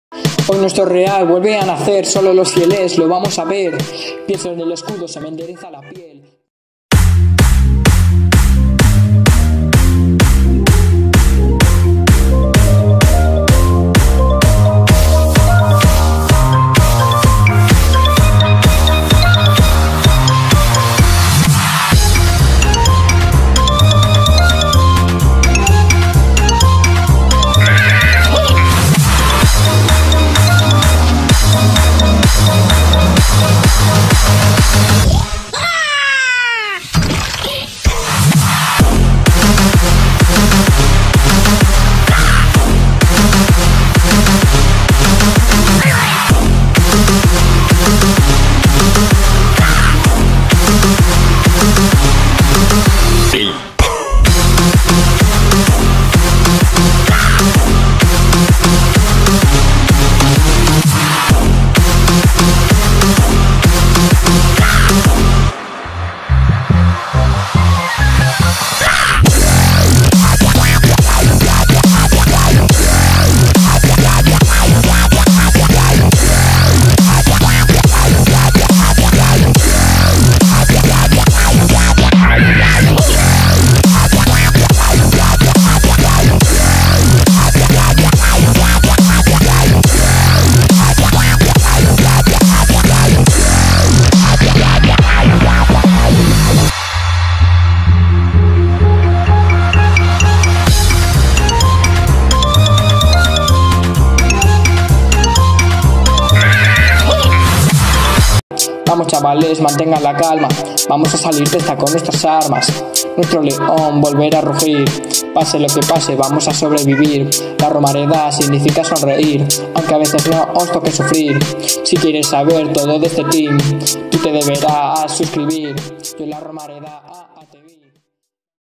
CD Numancia 1 VS Athletic Club 1
Nuevo amistoso para el equipo Bilbaino frente al Numancia de segunda división. Un empate a 1 que no dejo buenas sensaciones a ninguno de los dos equipos.